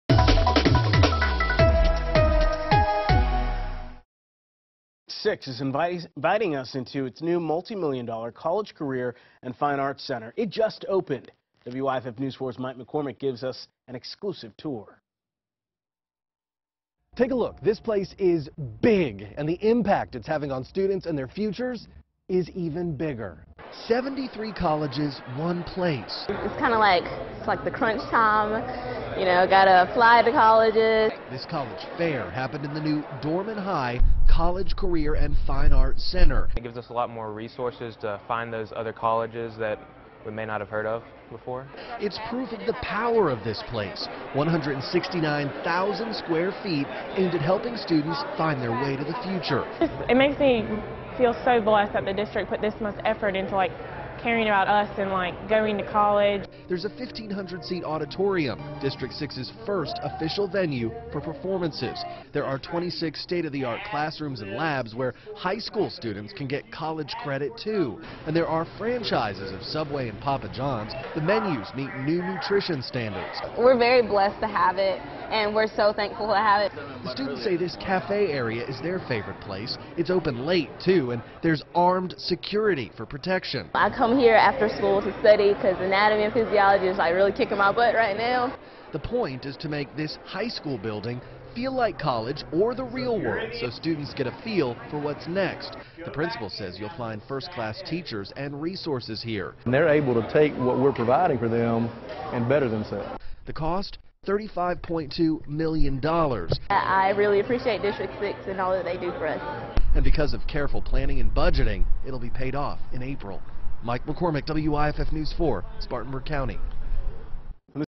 Spartanburg school district hopes new building helps students prepare for college
Spartanburg school district 6 opens new college prep and fine arts center.